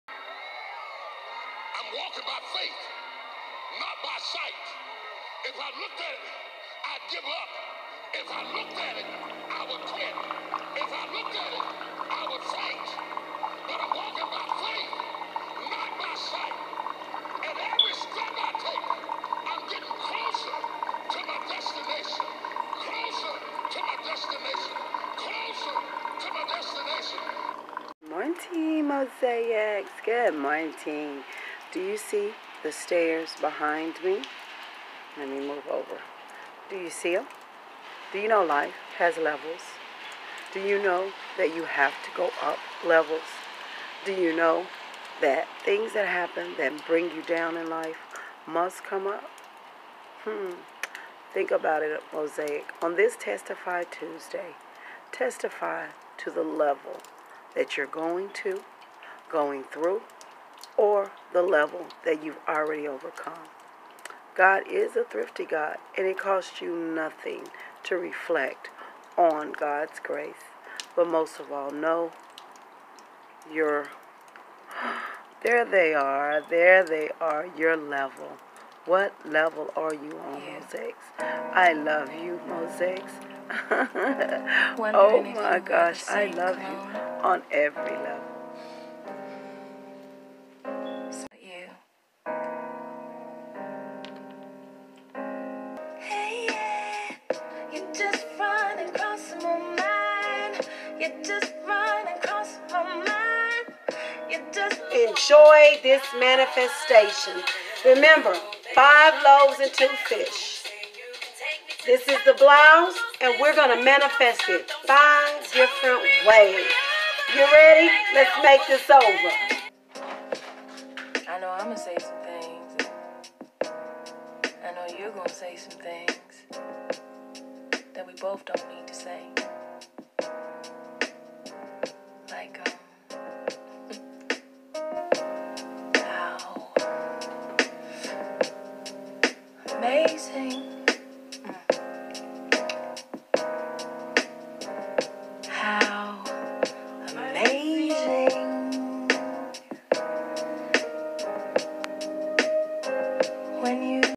Thrift Moments: Level Up...What "LEVEL" are you on???
Thrift Shirt Flip Plus Size Style Makeover